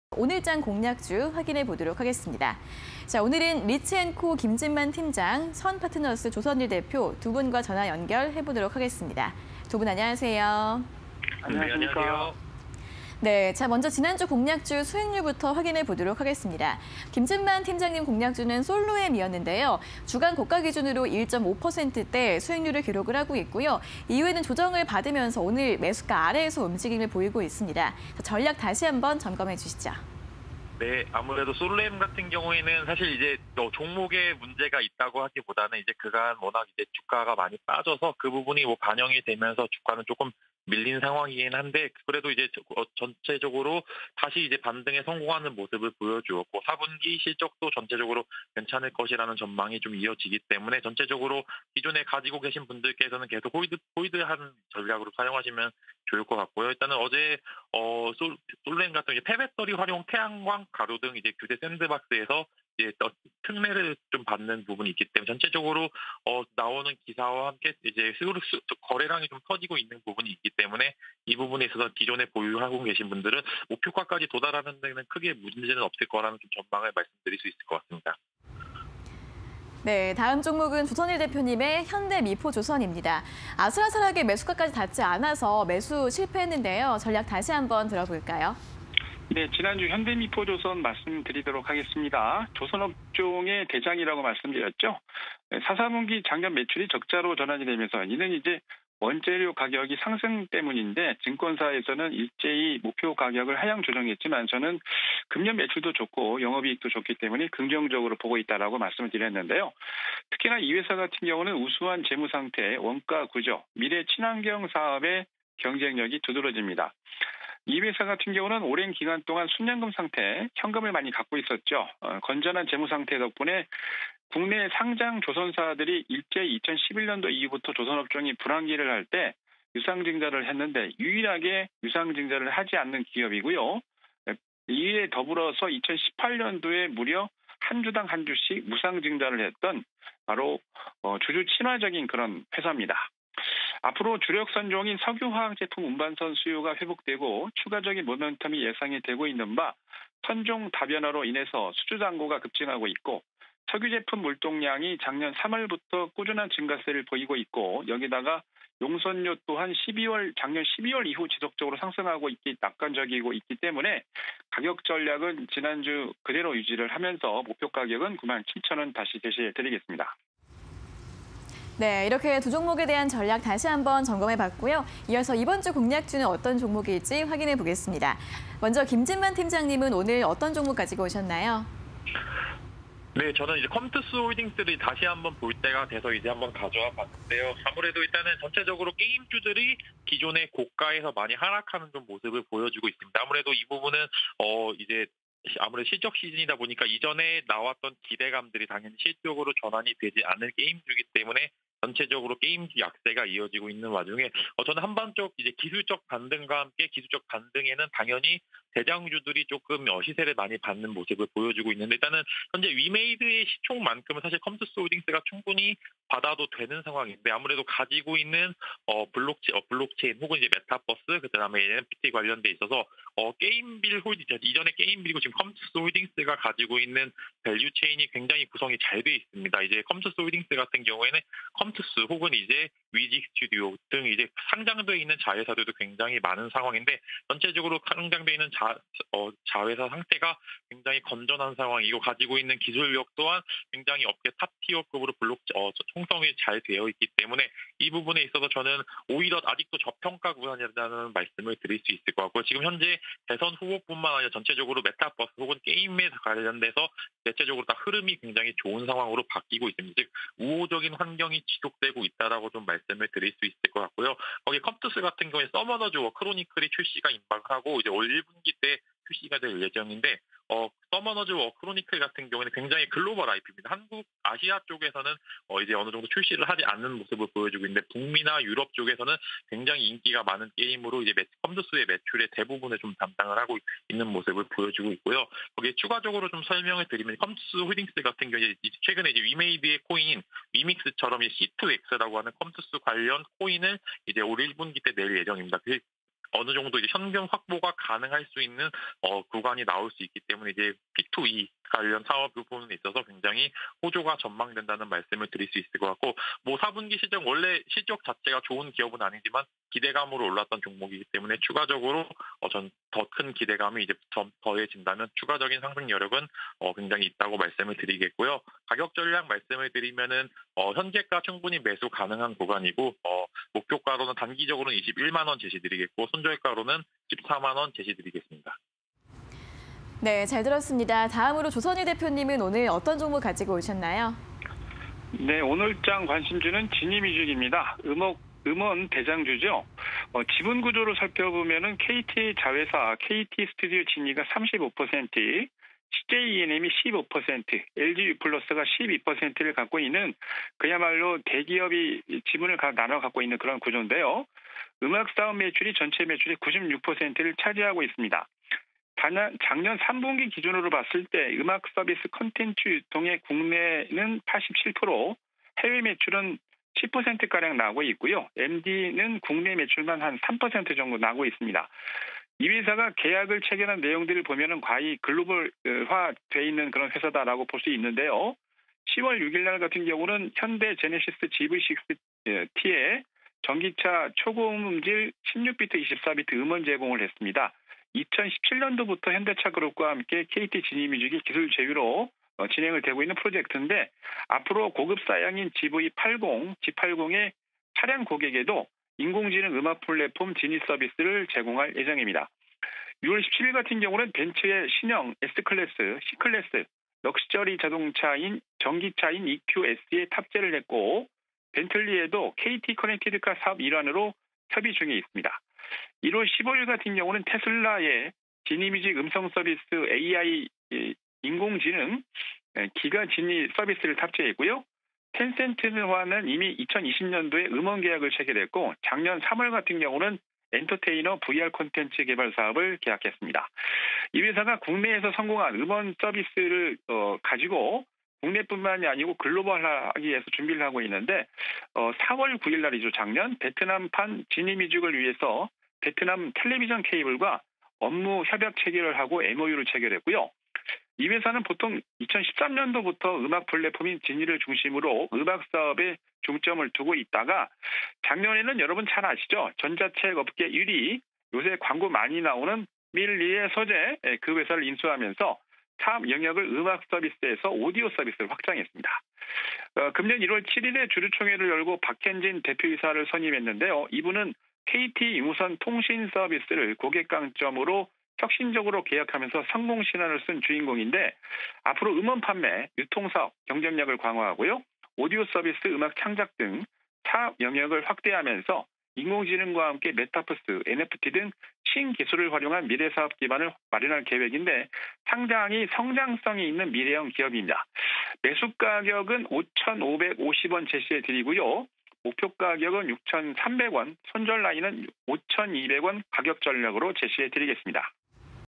컴투스홀딩스(063080) VS 지니뮤직(043610)_종목 VS 종목 (20220112)
컴투스홀딩스(063080)
- 대선 후보들, 게임업 관련 긍정적 의견
- ‘게임-엔터-코인’ 밸류체인 보유
- 국내 탑티어의 게임 지주회사
- 컴투스 ‘서머너즈워 크로니클’ 출시 임박
- 자체 개발 신작 ‘포커 타워 디펜스’ 글로벌 출시
- NFT 기반 잠재력 유효

지니뮤직(043610)
- 음악사업이 전체 매출의 96.7% 차지
- 지난해 3분기 음악 콘텐츠 유통 매출 1,585억 기록
- 제네시스 GV60 전기차에 초고음질 음원 제공
- 2017년부터 ‘현대차-KT-지니뮤직’ 기술 제휴
- 전기차 구매 고객들에 지니 서비스 제공 예정
- 벤츠 등과도 제휴… 음원 서비스 탑재
- 베트남 텔레비전 케이블과 업무협약 체결
- 전자책 업체 ‘밀리의 서재’ 인수… 사업영역 확장
- 박현진 대표이사 선임… 확장·혁신 박차
- NFT 활용 등 미래사업 기반 마련 계획